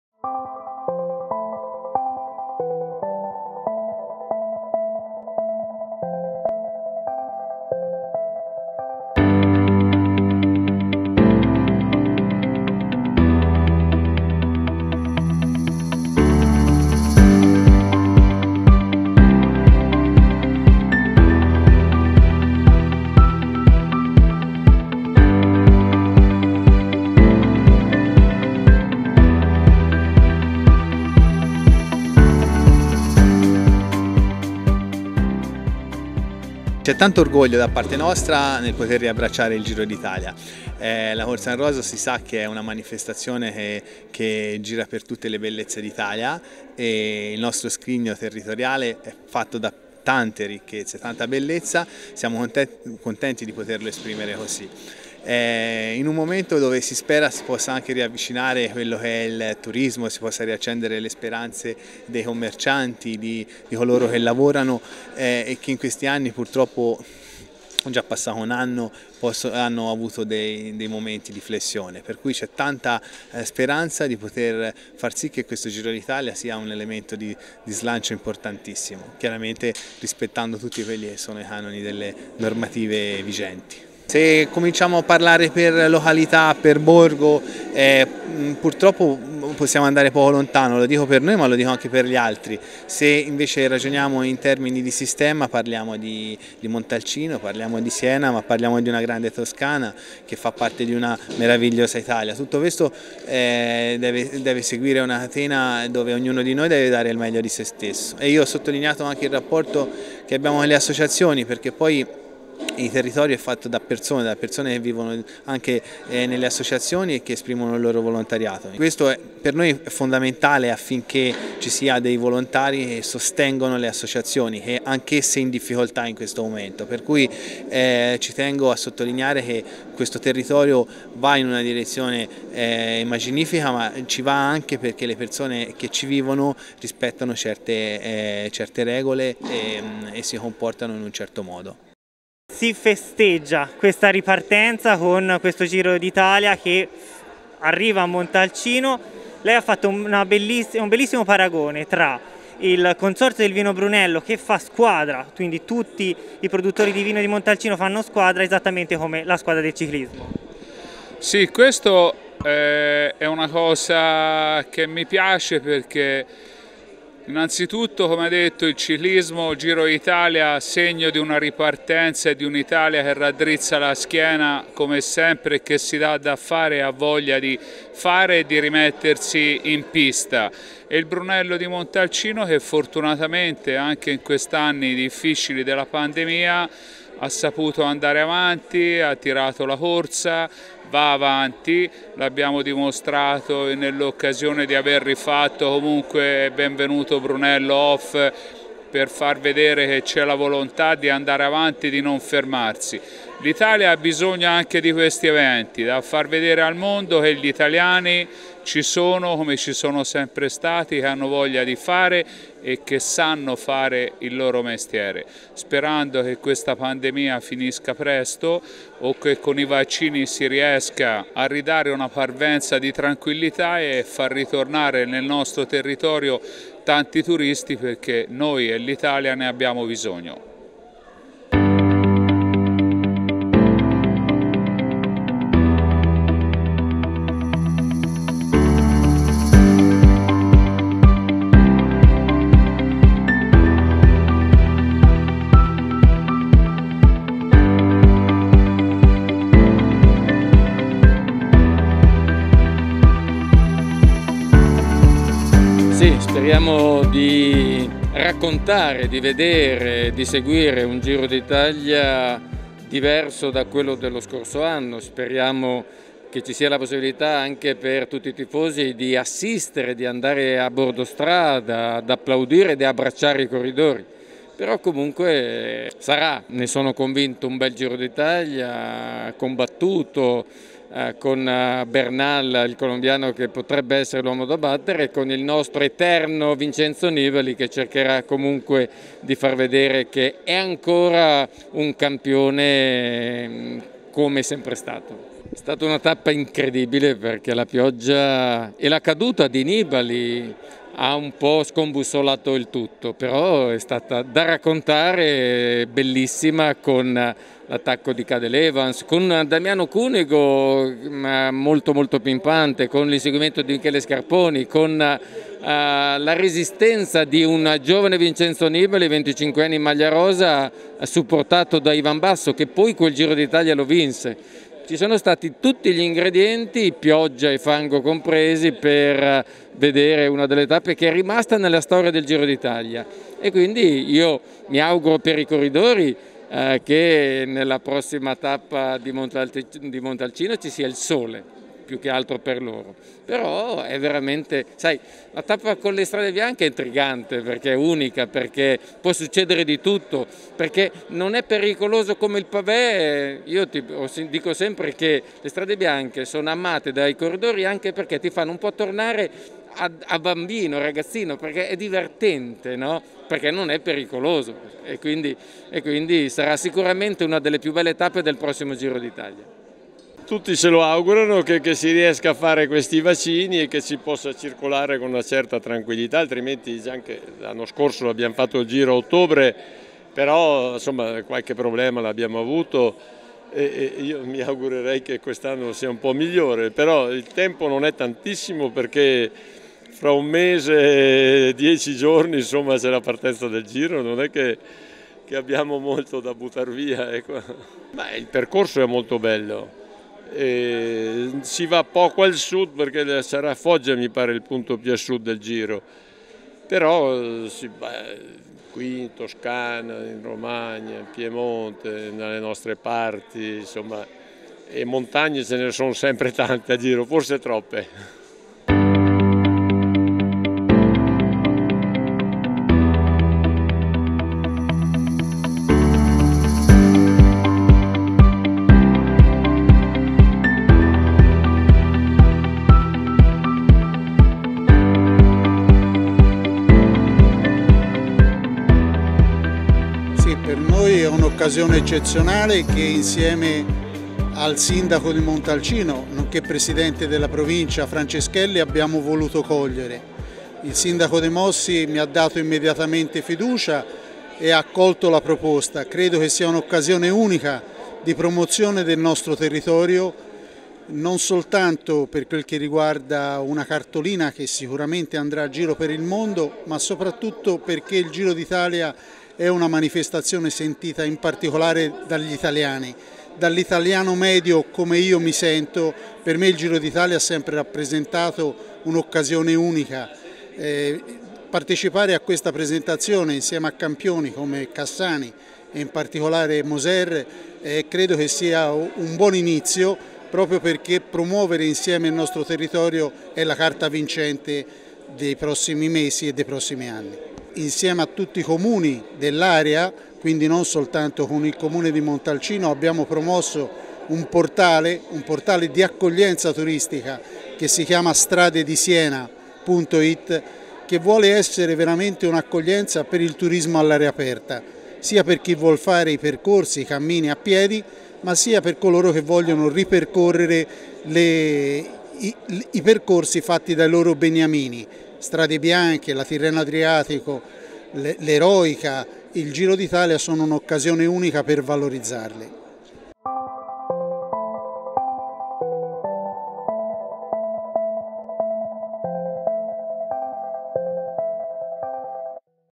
PRESENTAZIONE TAPPE GIRO D'ITALIA 2021 NELLE TERRE DI SIENA
- ANGELO BRACONI, VICESINDACO DI MONTALCINO
- FABRIZIO BINDOCCI, PRESIDENTE CONSORZIO VINO BRUNELLO DI MONTALCINO
- DAVIDE CASSANI CT NAZIONALE CICLISMO
- FRANCESCO MOSER, CAMPIONE CICLISMO
- ALBERTO TIRELLI, ASSESSORE AL TURISMO DEL COMUNE DI SIENA